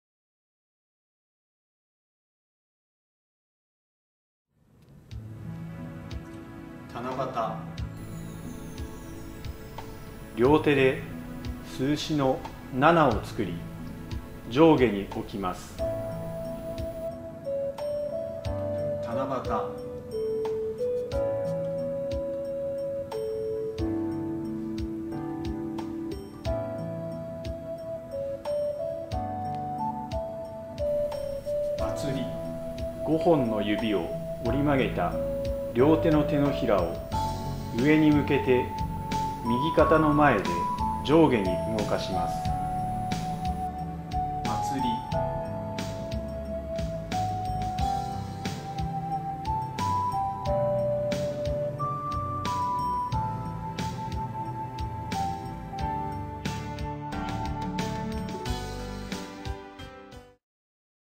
ちょこっと手話No.37
七夕
　両手で数詞の「7」を作り、上下に置きます。

祭り
　5本の指を折り曲げた両手の手のひらを上に向けて右肩の前で上下に動かします。